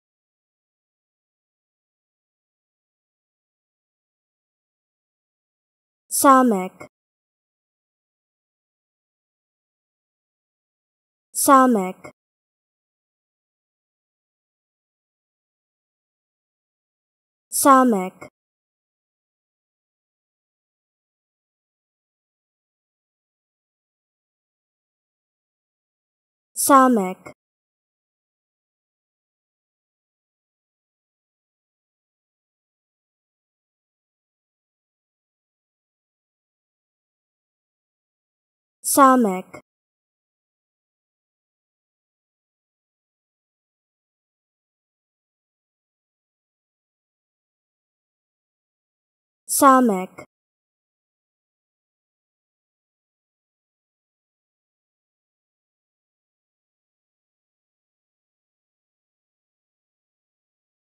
How To Say Samekh
Learn how to say Samekh with EmmaSaying free pronunciation tutorials.